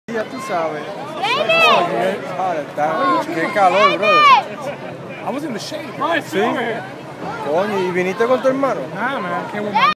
Johnny Damon sighing autographs
Johnny Damon of the Tampa bay rays was sighing autographs after the rays win against the Florida marlins in Miami.